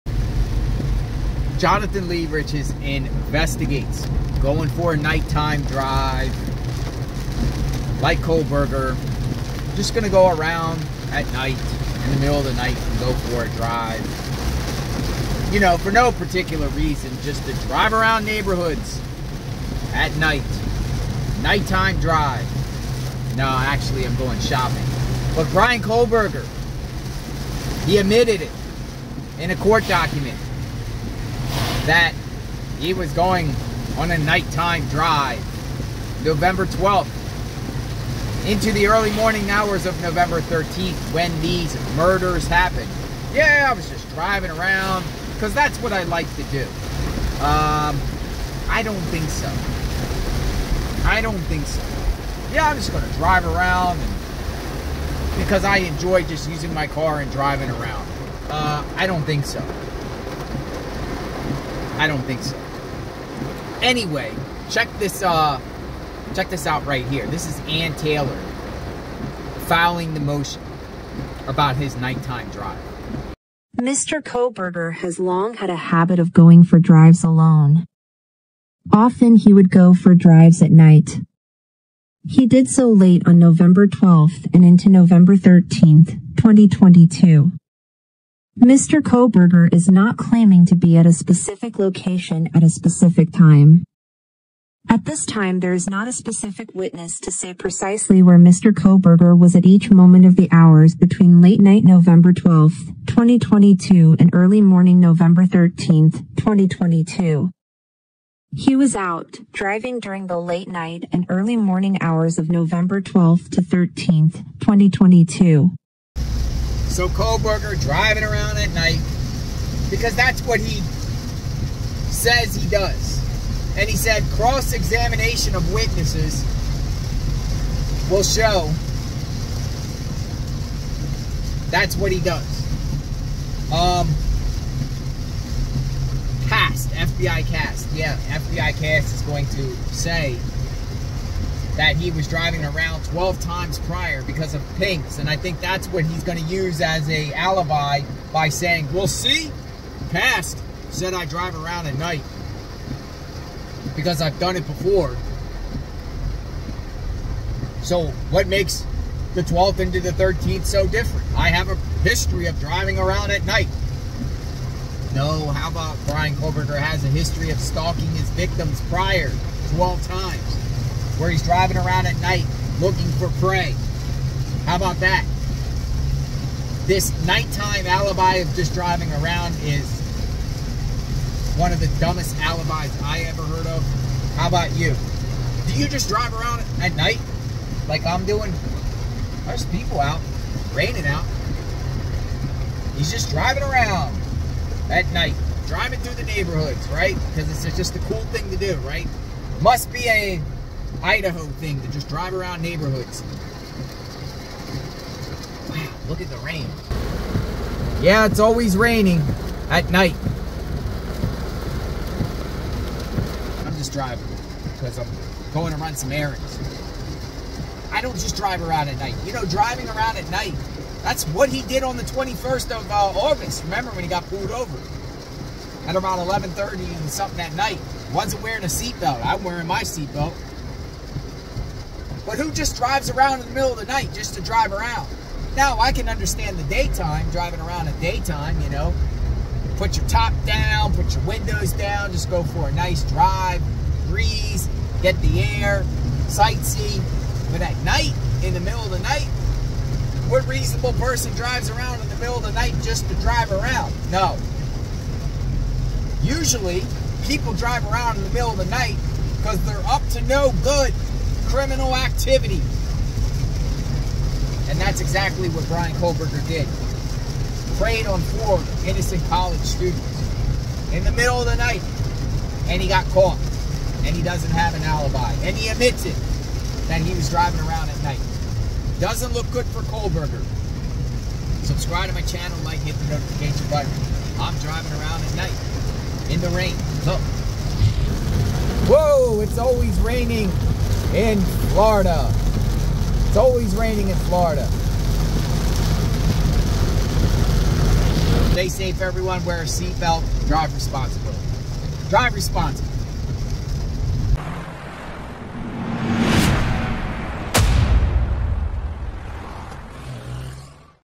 Bryan Kohberger! HE ADMITS IT.
I am covering the Moscow Idaho / Bryan Kohberger case.